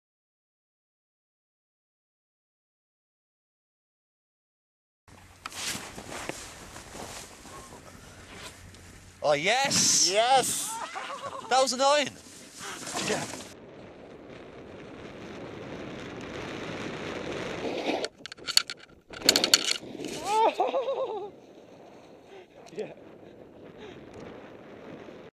Snowboard: My First Ever Cab 900
David Raybould, Steinplatte, Austria.

Filmed on video camera and headcam. Both edited together.

You can hear how shocked I am to land it. :)